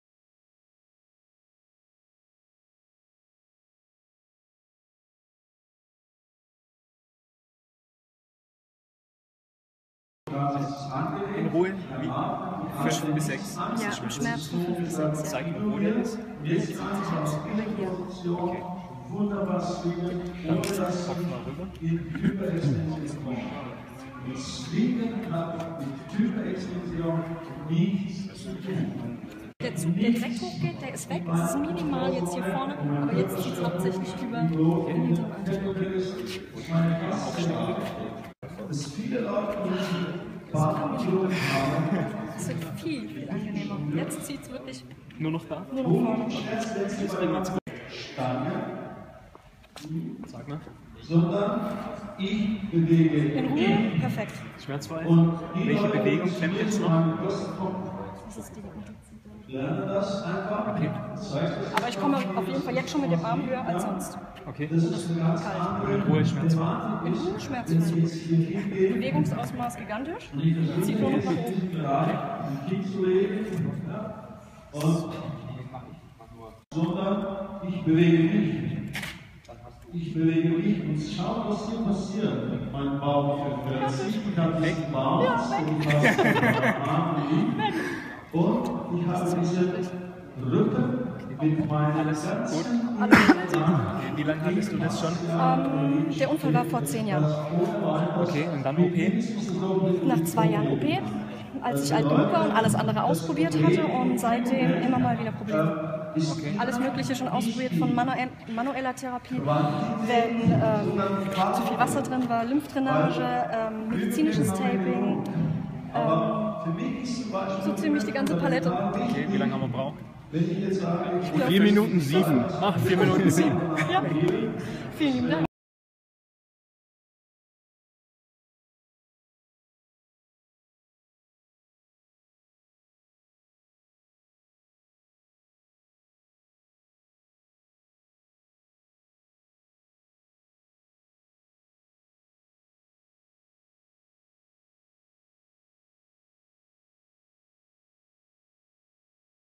Fascia Summit Munich 2015 - Myofascial Taping - Markus Erhard - Video #1
Myofascial Taping - Live demo for shoulder pain.
From pain 5-6 (0 = no pain, 10 = max pain) to pain free within stripes of tape.